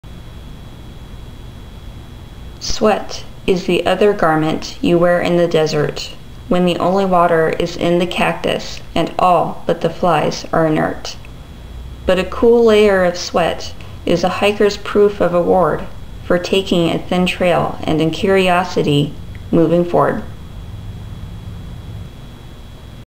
Sweat is the other garment, A Poem
Poem and Photo copyrighted by Amy Michelle Mosier. From my ebook, Envious of the Clouds, available @ Amazon. The photo is of Cathedral Rock in Sedona, AZ.

Sweat is the other garment
You wear in the desert
When the only water is in the cactus
And all but the flies are inert

But a cool layer of sweat
Is a hiker's proof of award
For taking a thin trail
And in curiosity, moving forward.